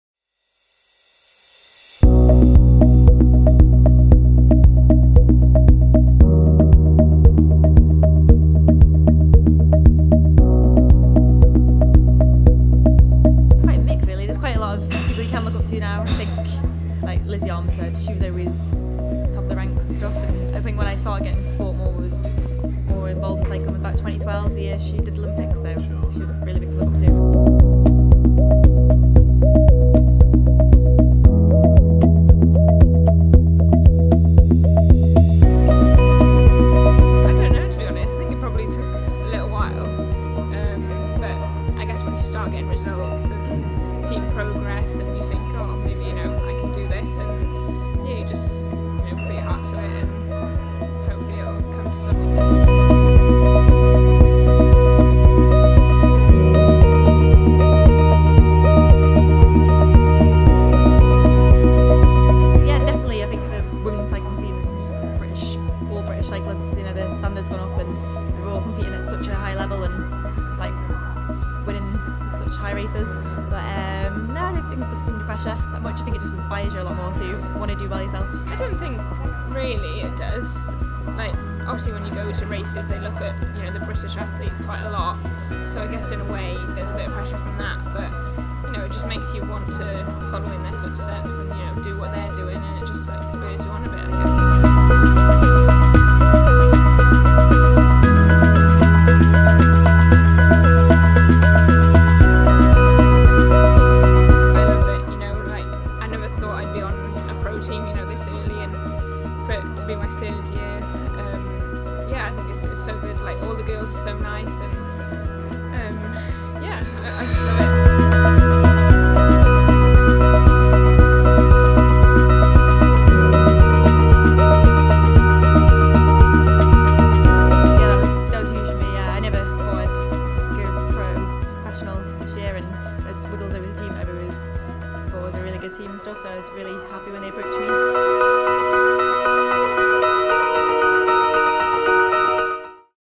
Wiggle Honda's Anna Christian And Amy Roberts Talk Teammates, Inspirations And GB Pressure
Wiggle Honda riders Anna Christian and Amy Roberts speak exclusively to Sportsvibe about joining one of their sport's biggest teams, who inspired them to get into cycling and the pressure of competing as a GB athlete.